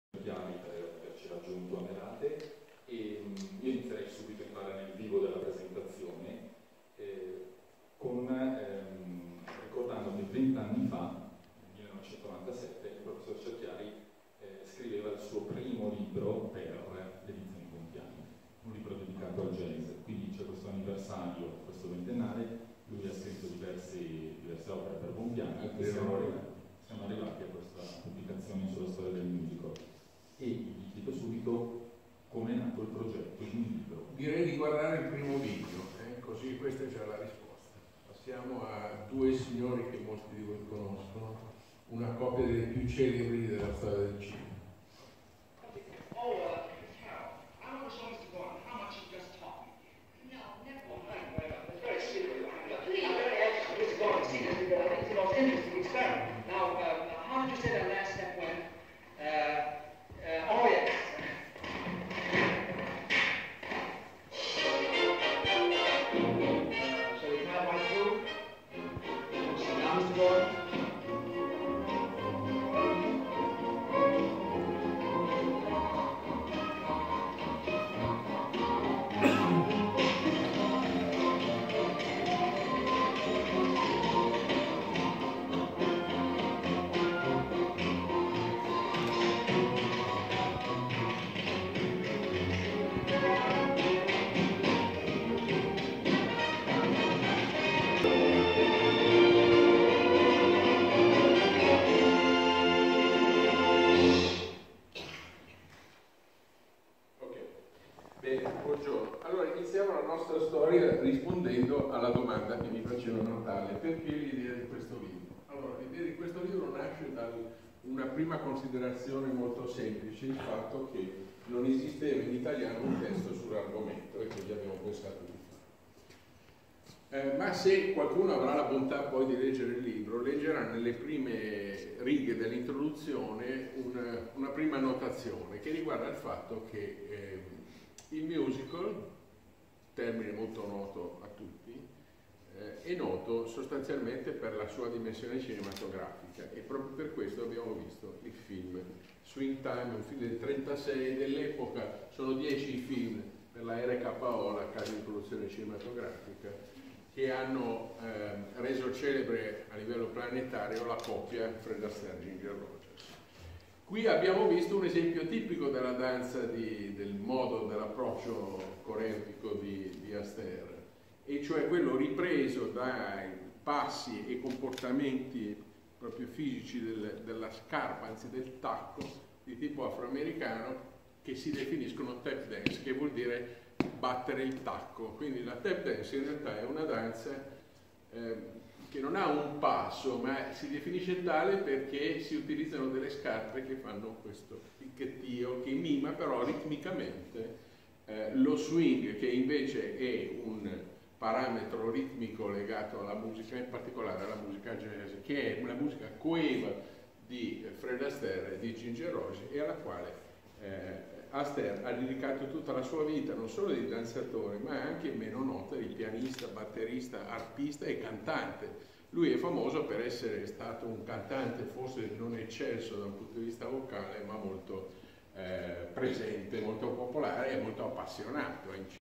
Merate, presentazione libro Storia del Musical
Di Luca Cerchiari, professore universitario della IULM, e Andrea Natale. Evento all'interno della rassegna ''Merate Musica''.